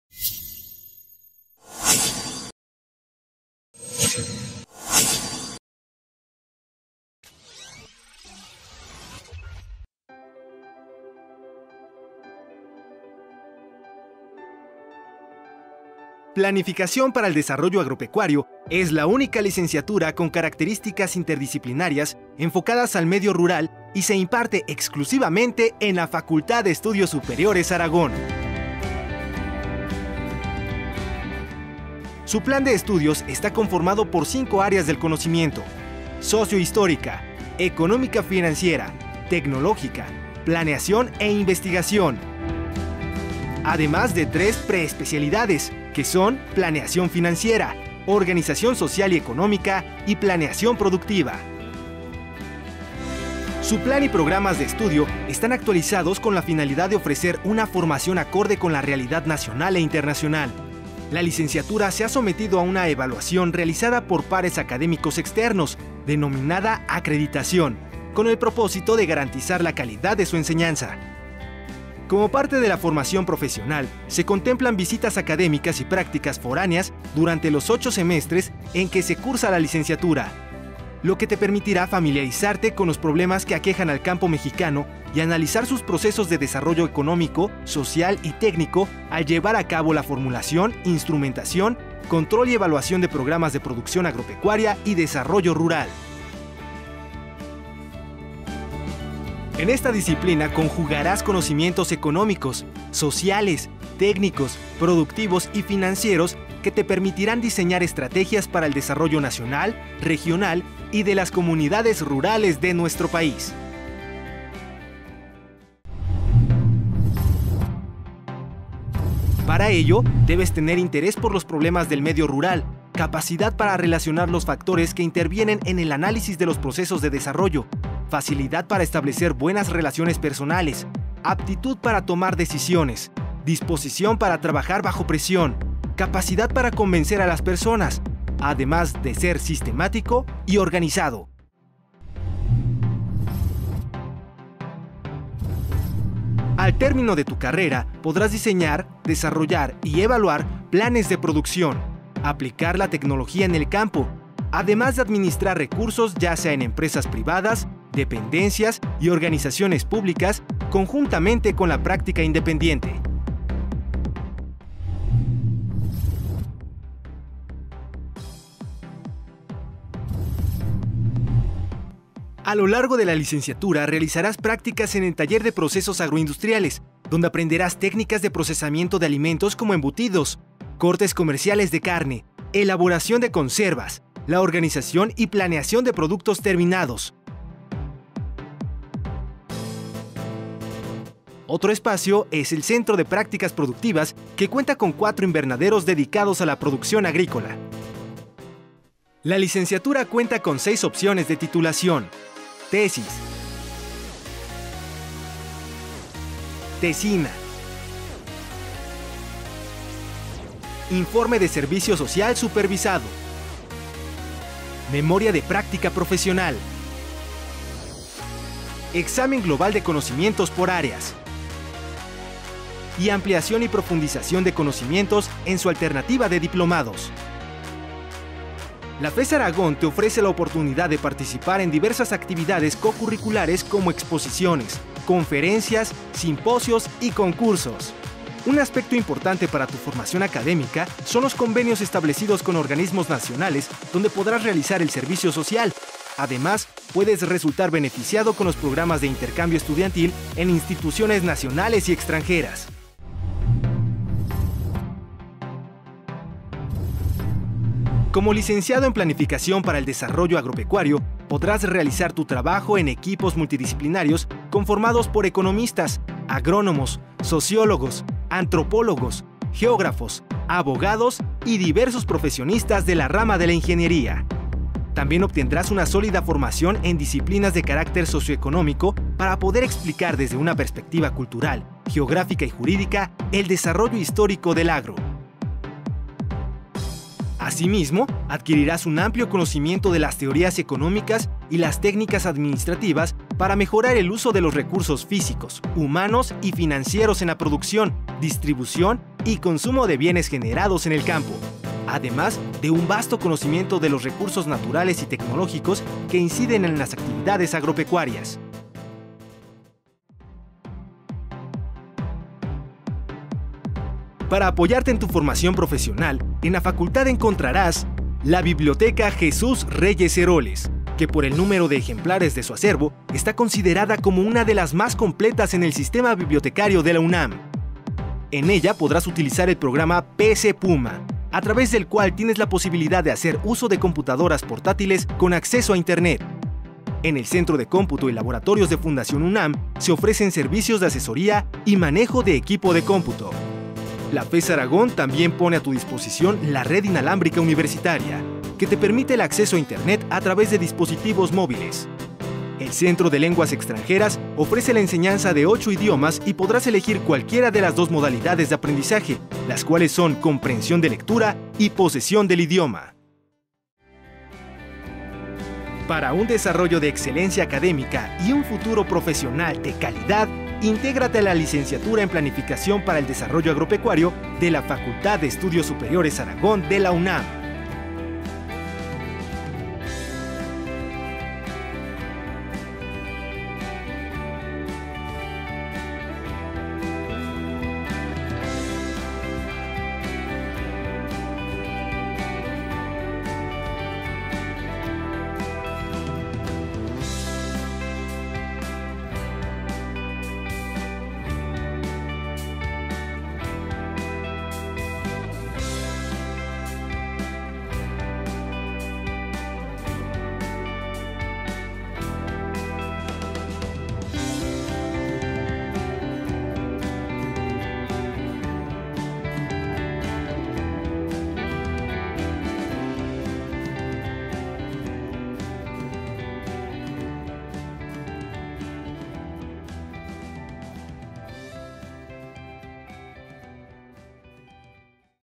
PLANIFICACIÓN PARA EL DESARROLLO AGROPECUARIO
La licenciatura en Planificación para el Desarrollo Agropecuario, que está instrumentada dentro del sistema UNAM, es la única a nivel licenciatura con características interdisciplinarias enfocadas al medio rural. Se imparte exclusivamente en la FES Aragón. Su plan de estudios está conformado por cinco áreas del conocimiento: Sociohistórica, Económico-Financiera, Tecnológica, Planeación e Investigación. Para una mejor formación profesional, dentro de la currícula, se contemplan visitas académicas y prácticas foráneas en el transcurso de los semestres y al final de ellos, las cuales permiten a los alumnos familiarizarse con los problemas que aquejan al campo y analizar los procesos de desarrollo económico, social y técnico con relación a las condiciones sociales y económicas de la población rural; llevando a cabo la formulación, instrumentación, control, evaluación y programas de producción agropecuaria y desarrollo rural.